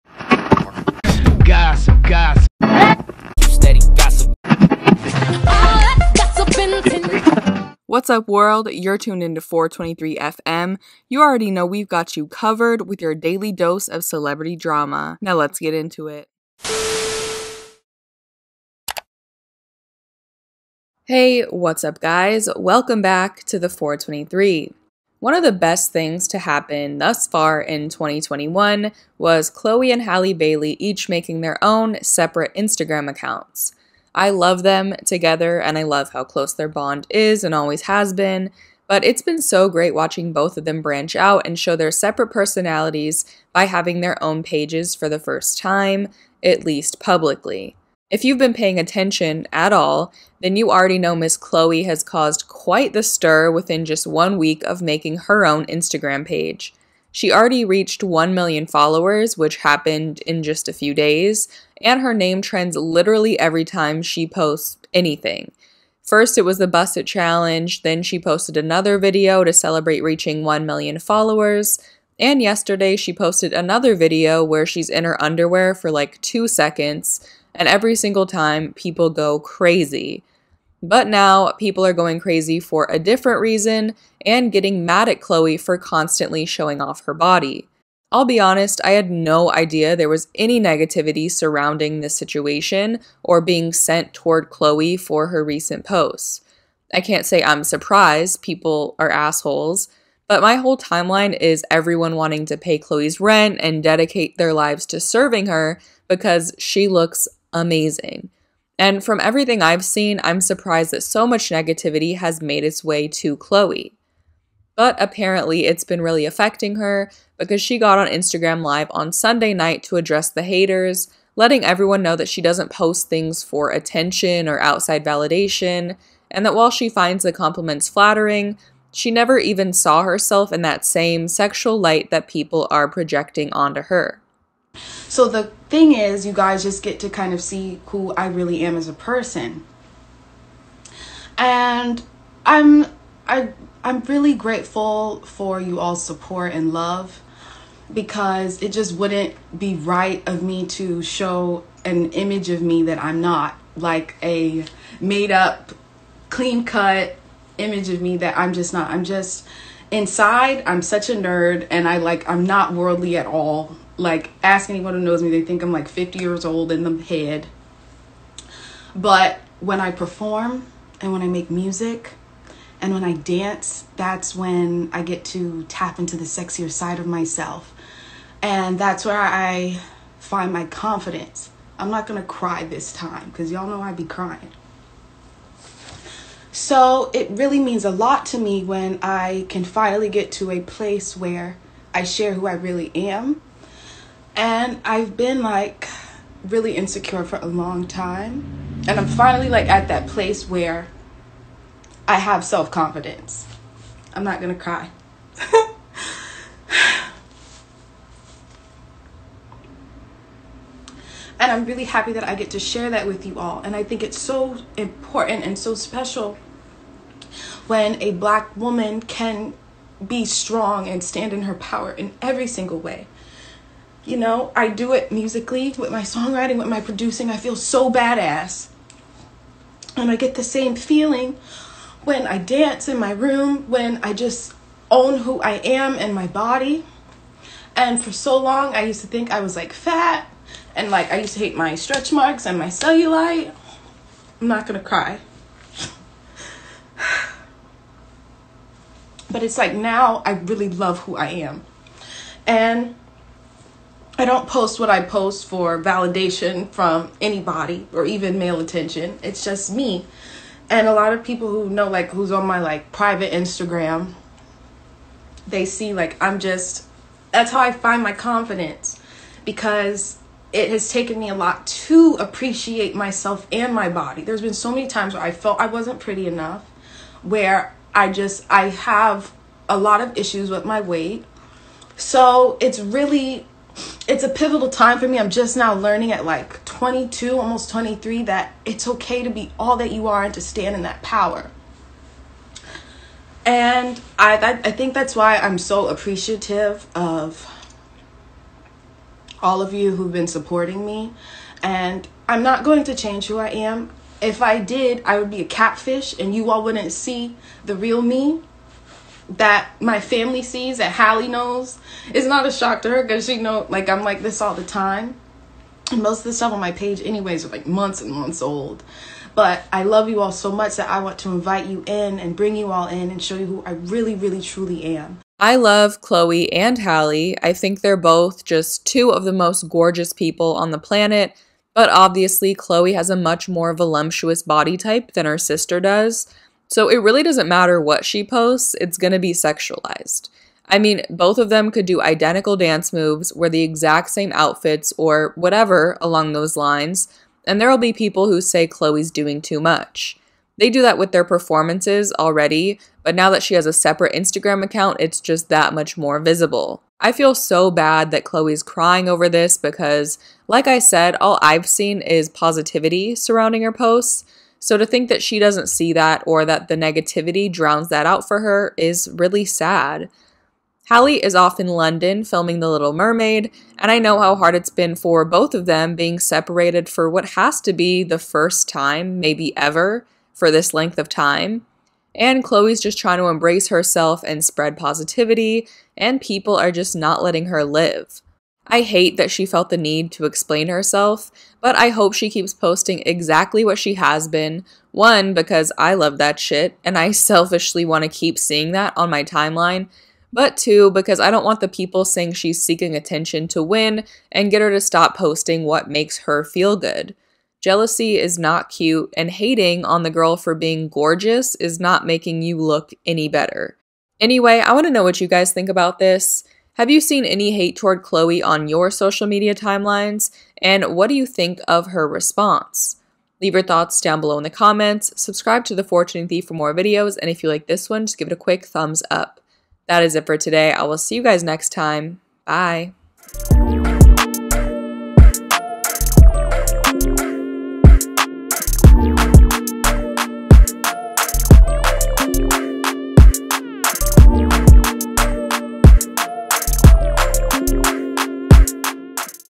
Chloe Bailey Cries Over Online Hate After Successful IG Launch
Hiiiiiii chismosas 🧡

In today’s video, I talk about Chloe Bailey (one half of Chloe x Halle) going on Instagram live to address the people sending her hate over her recent Instagram posts.

LET ME KNOW DOWN BELOW IN THE COMMENTS  → Have you seen anyone sending negativity her way?? What do you think of her response?

FOLLOW ME

© The Four23 by Rebecah Jacobs